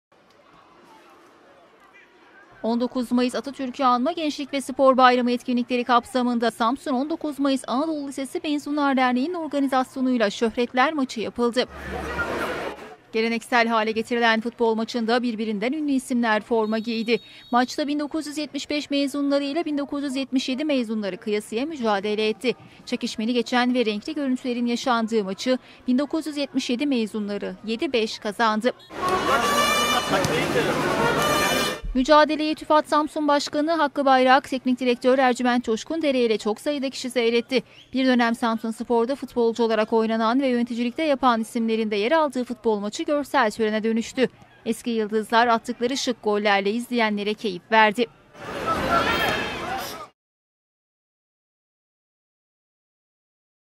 ŞÖHRETLER BÜYÜLEDİ
Samsun 19 Mayıs Anadolu Lisesi Mezunları Derneği tarafından geleneksel hale getirilen futbol maçında ünlü isimler forma giydi. Şöhretler,  maç boyunca attıkları şık gollerle izleyenlere keyif verdi.